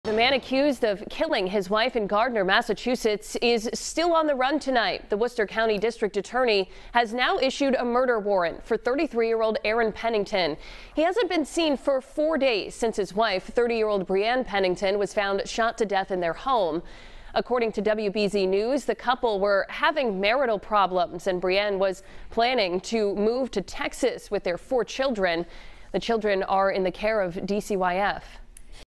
Murder warrant issued as hunt for victim's husband continues
The Worcester County District Attorney’s Office issued a murder warrant for Aaron Pennington in Gardner District Court on Wednesday.